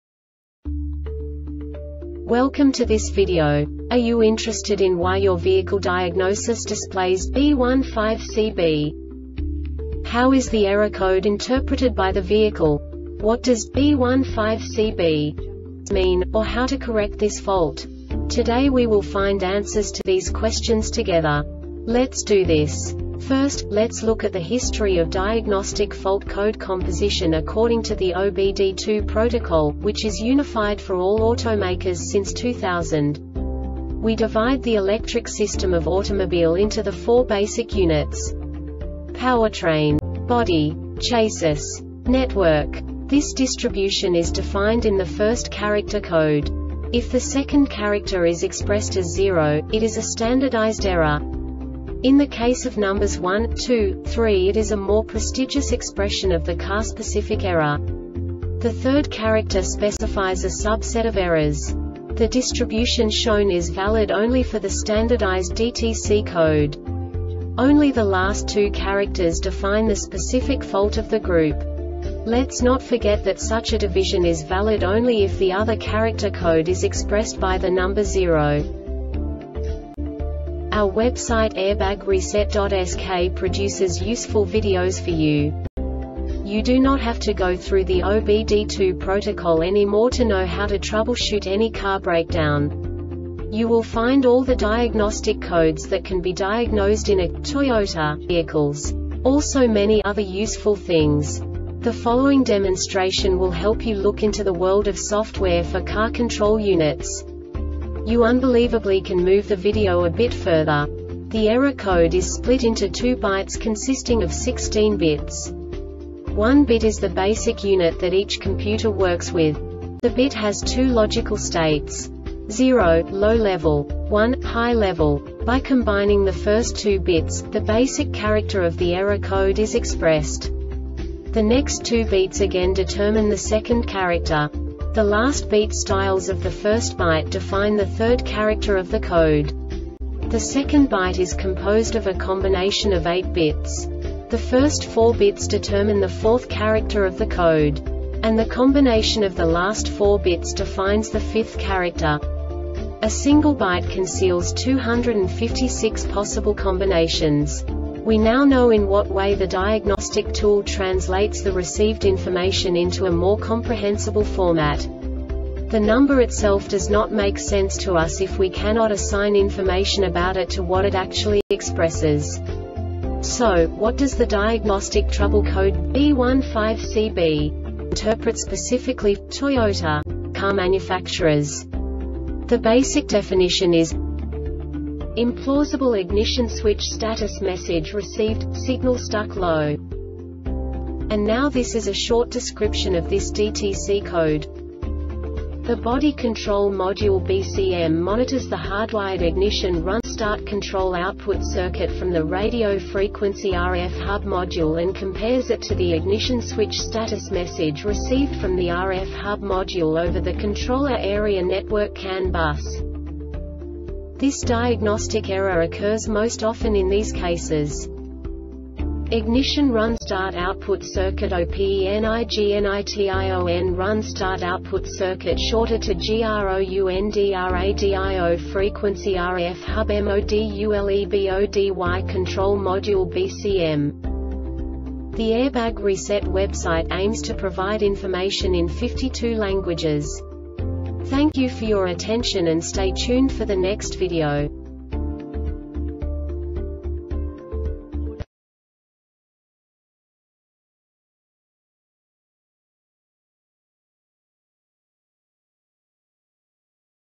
DTC Toyota B15CB Short Explanation
Contents:
0:21 Basic DTC analysis according to OBD2 protocol standard.
1:48 Insight into programming
2:58 A diagnostically understandable description of the Diagnostic Trouble Code
3:05 Basic error definition

Make:
Toyota
Code:
B15CB
Definition:
Implausible Ignition Switch Status Message Received - Signal Stuck Low
Description:
The Body Control Module (BCM) monitors the hardwired Ignition Run or Start Control Output circuit from the Radio Frequency (RF) Hub Module and compares it to the ignition switch status message received from the RF Hub Module over the Controller Area Network (CAN) Bus.
Cause:
IGNITION RUN-START OUTPUT CIRCUIT OPENIGNITION RUN-START OUTPUT CIRCUIT SHORTED TO GROUNDRADIO FREQUENCY (RF) HUB MODULEBODY CONTROL MODULE (BCM)